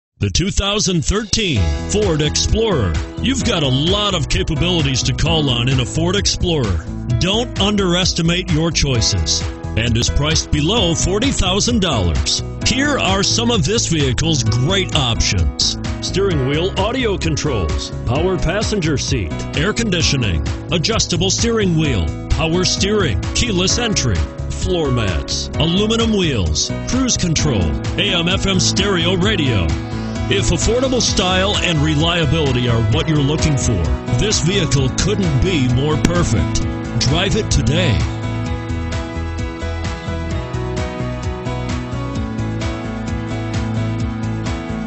2013 FORD EXPLORER Taylor, MI C14409
2013 FORD EXPLORER Taylor, MI
Stock# C14409

Don't miss this 2013 Ford Explorer. It's equipped with Automatic transmission and features a Deep Impact Blue Metallic exterior. With 0 miles, you'll want to take this car home. Make a great choice today.